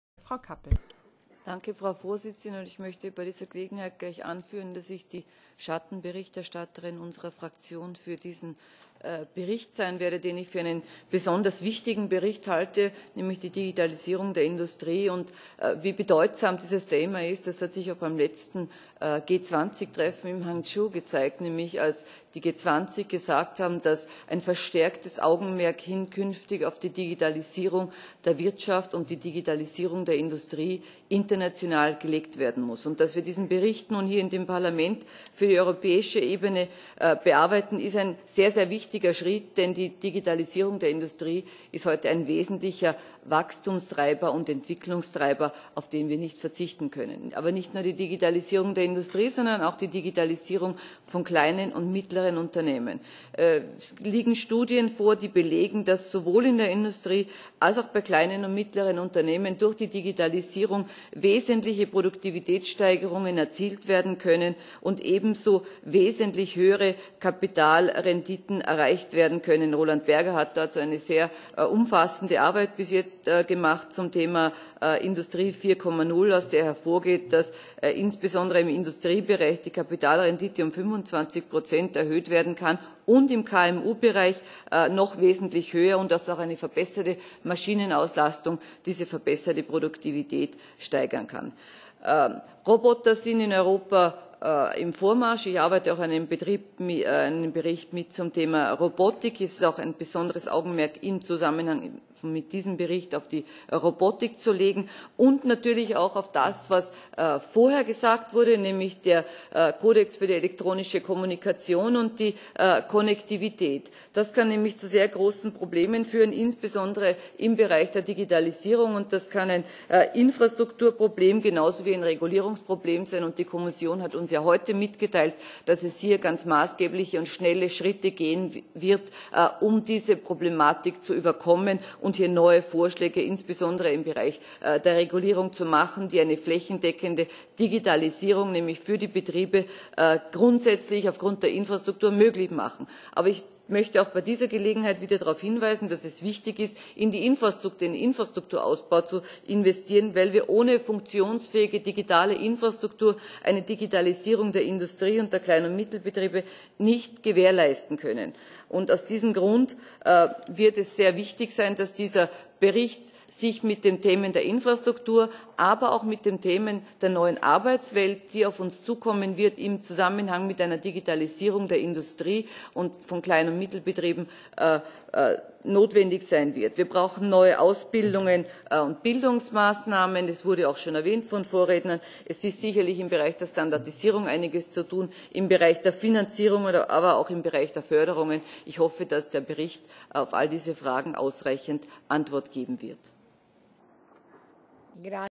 Barbara Kappel - Digitalisierung von Industrie und Infrastruktur (ITRE)
Ausschuss für Industrie, Forschung und Energie (ITRE), Ausschusssitzung vom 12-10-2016, Thema: Digitalisierung von Industrie und Infrastruktur